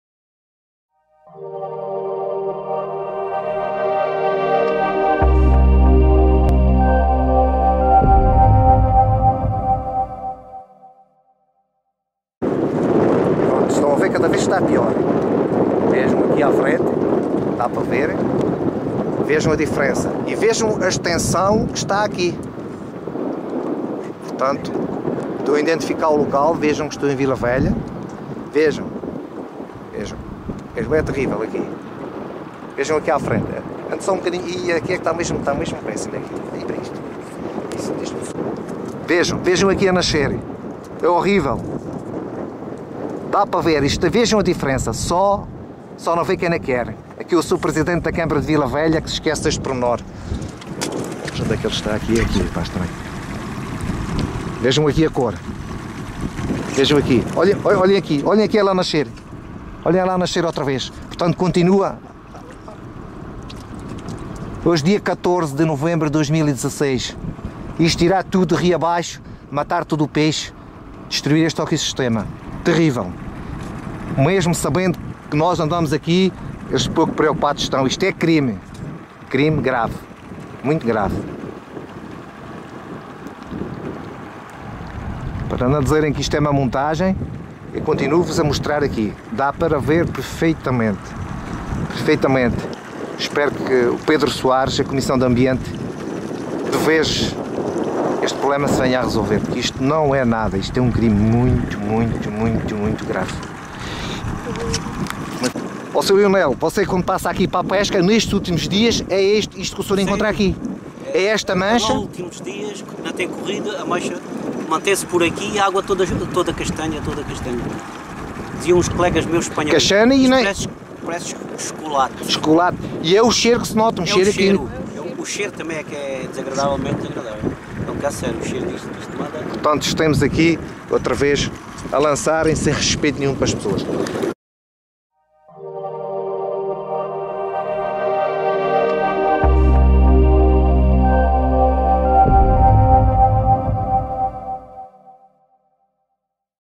PROTEJO DENUNCIA DESCARGA DE POLUIÇÃO NO RIO TEJO
O ambientalista Arlindo Marques, do movimento ProTejo, filmou na terça-feira, dia 14 de novembro, novas descargas poluentes no rio Tejo, que terão provocado a morte a milhares de lagostins junto ao cais do Arneiro, no concelho de Nisa.